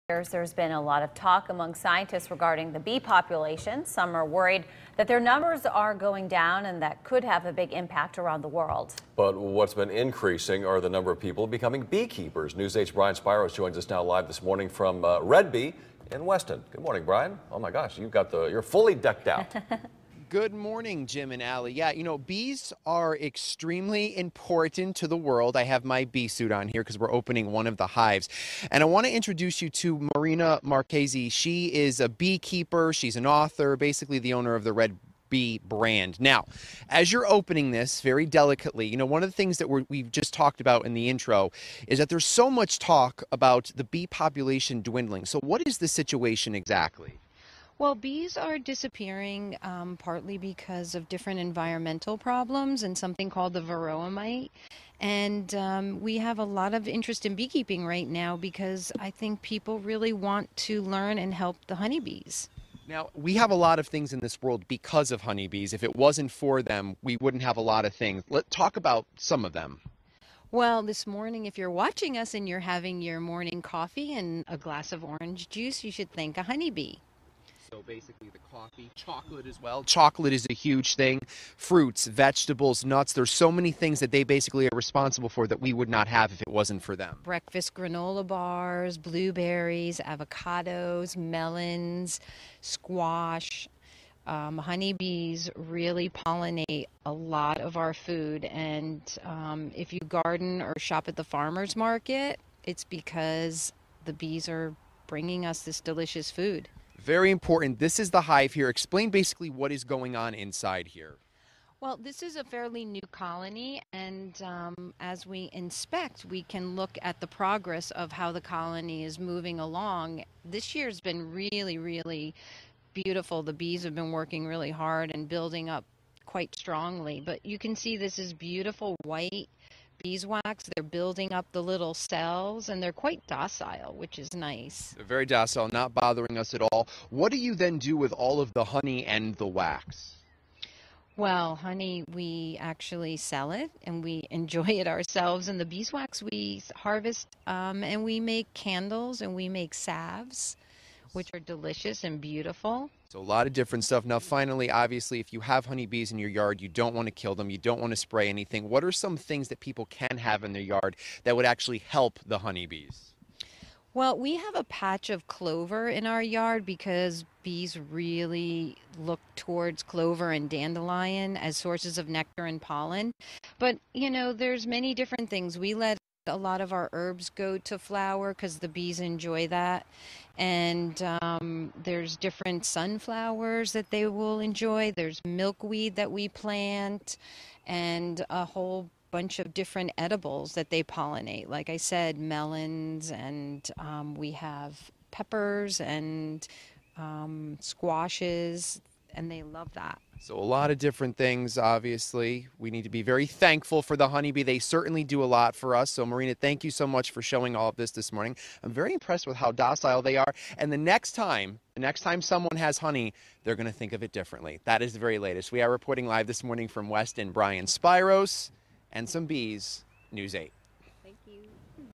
Beekeeper talks importance of bees for food, environment
In recent years there's been a lot of talk among scientists regarding the bee population.